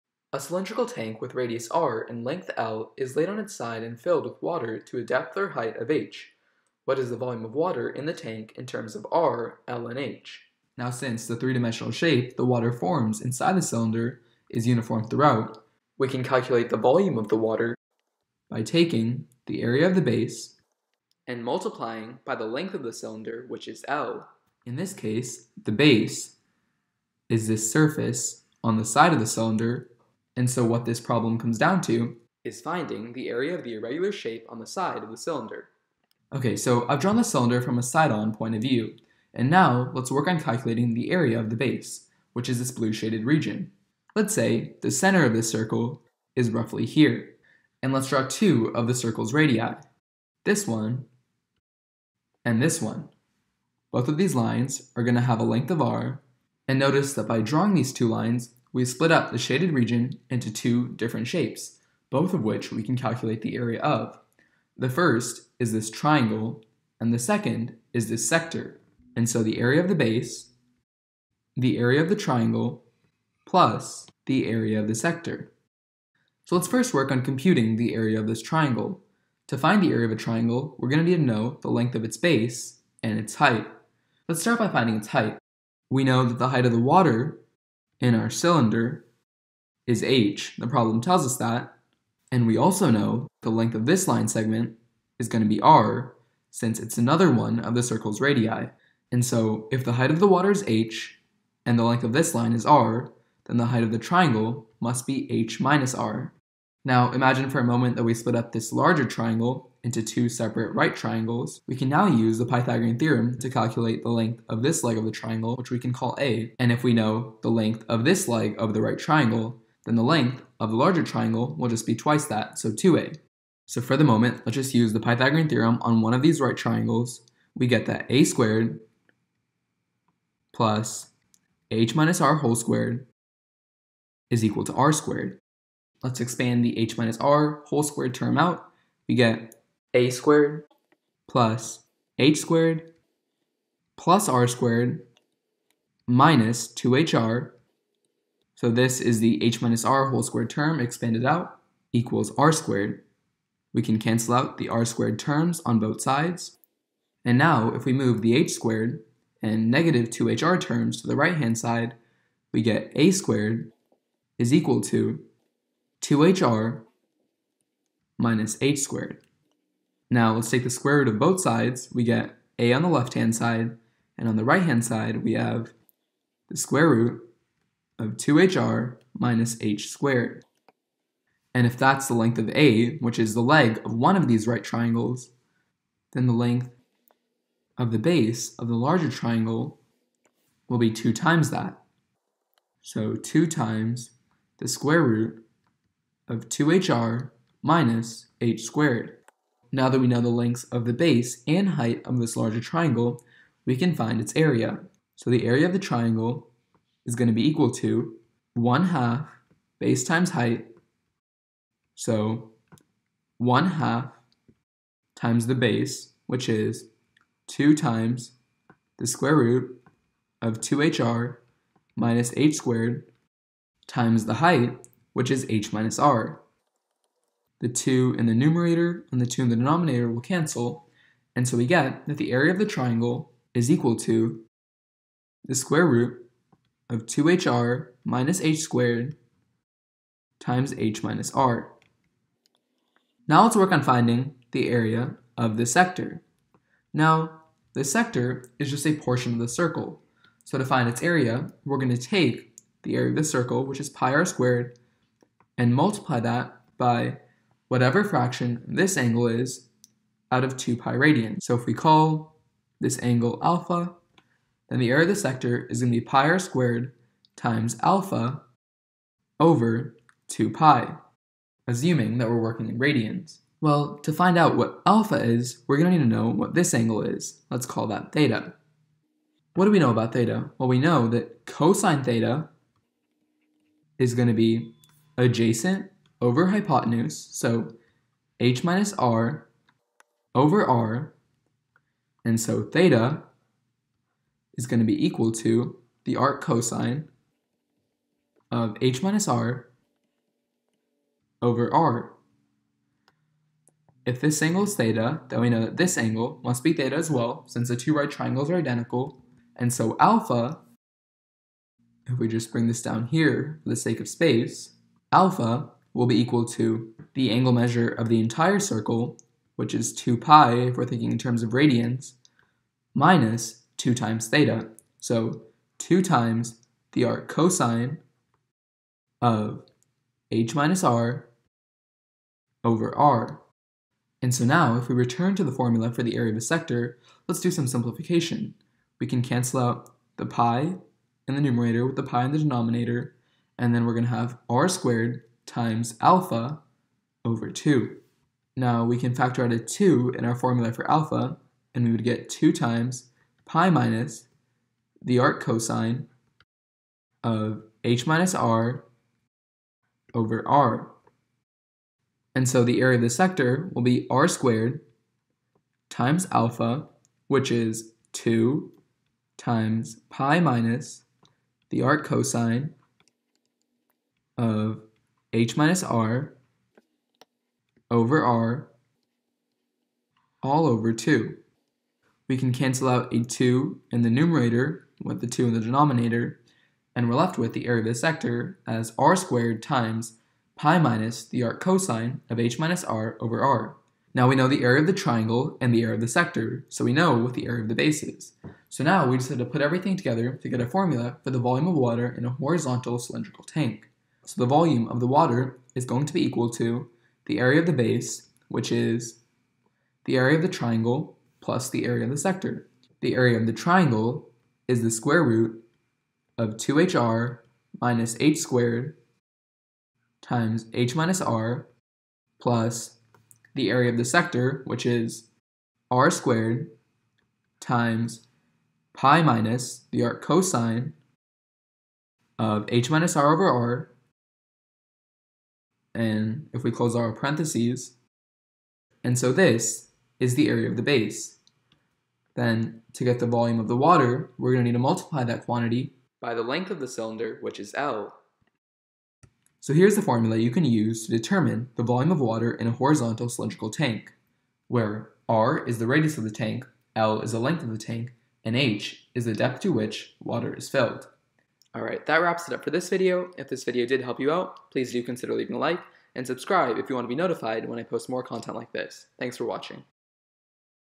Volume of Water in a Horizontal Cylindrical Tank (General Formula)
In this video, we derive a general formula for the volume of water in a cylindrical tank with radius r and length L that is laid on its side and filled with water to a depth/height of h inches.